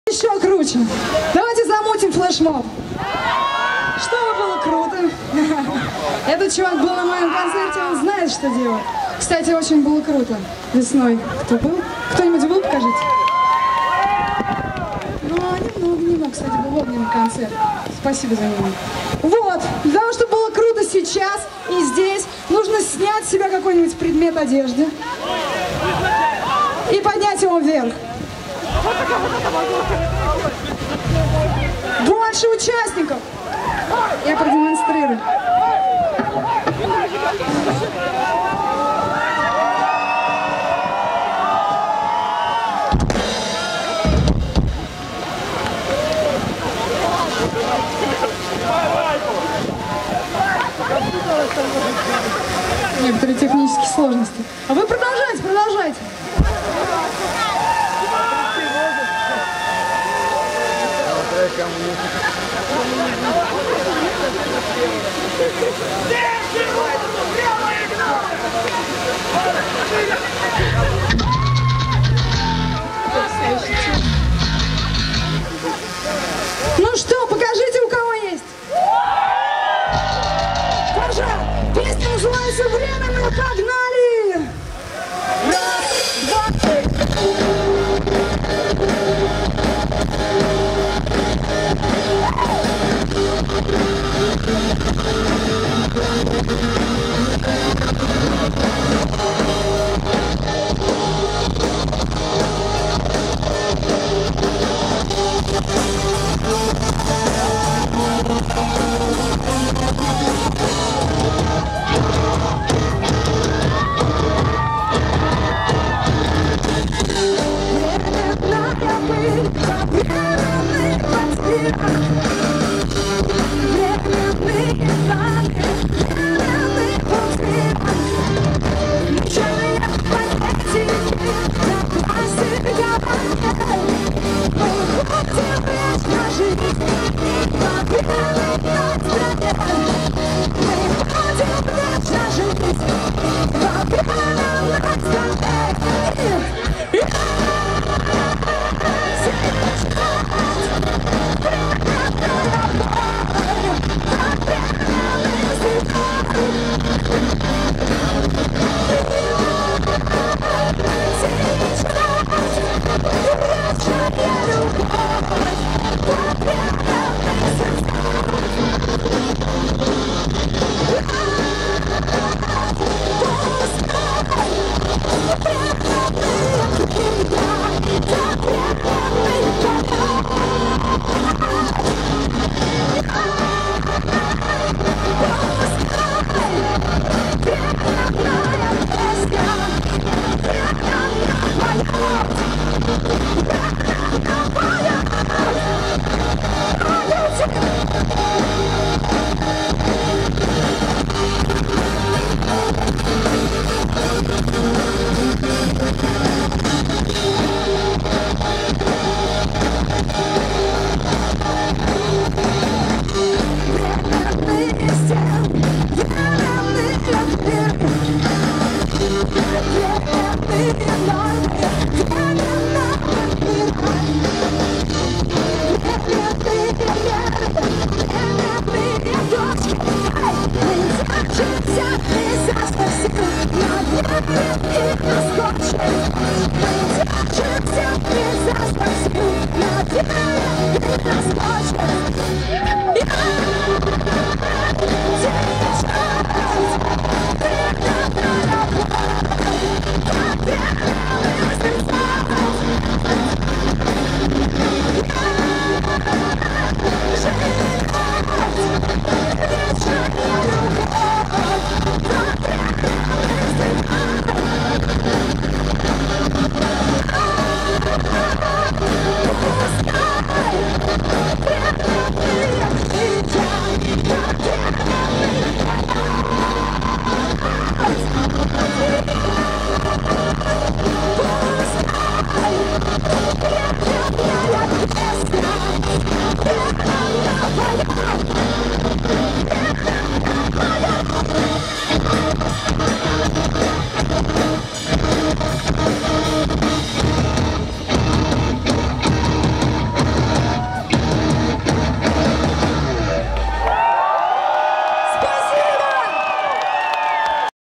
НУКИ
Рок-фестиваль "Калининград in Rock-2018" / Калининград / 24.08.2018